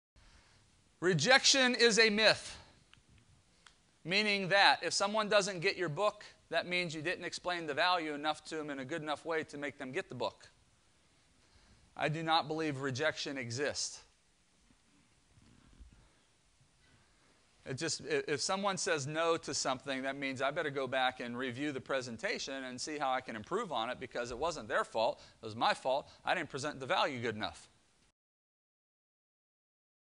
Motivational Speaker Tip: Rejection is a Myth
Keynote Speaker James Malinchak shares key tips for offering your product and getting to the cash fast!